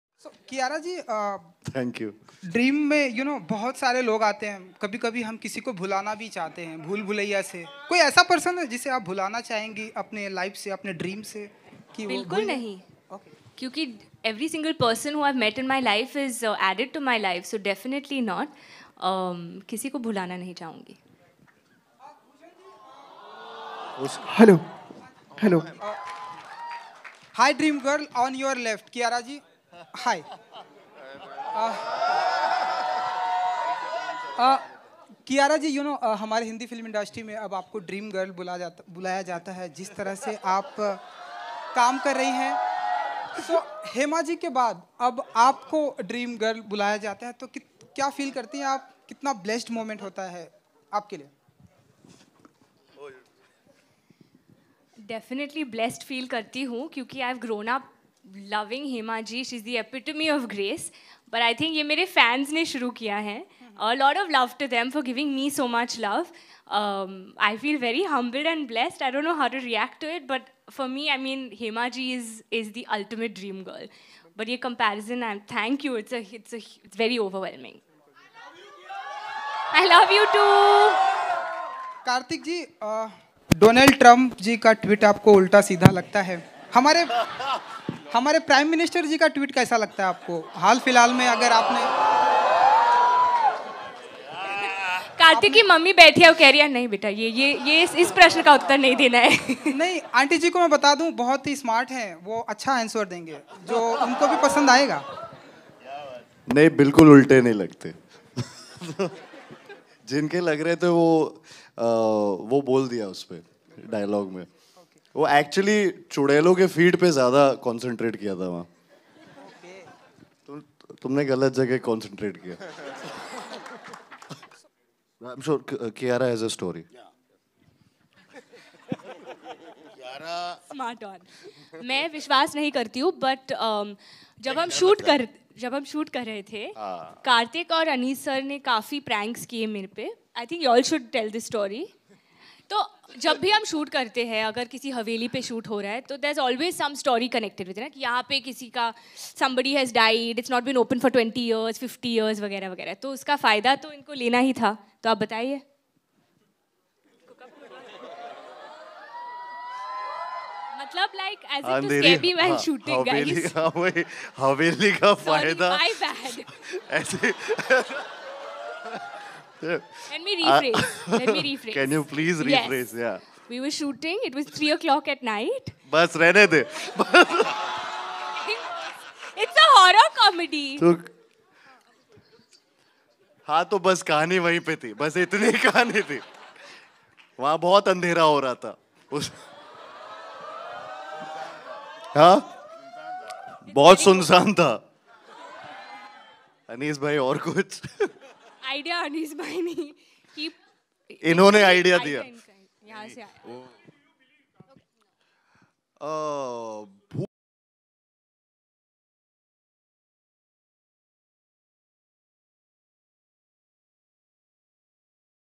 Kiara Advani Emotional After Breakup With Sidharth Malhotra At Bhool Bhulaiyaa 2 Movie Event
Kiara Advani Break Silence On Breakup With Boyfriend Sidharth Malhotra At Bhool Bhulaiyaa 2 Movie Interview Kartik Aaryan.

For Bollywood Latest Video Follow Us Official Instagram Account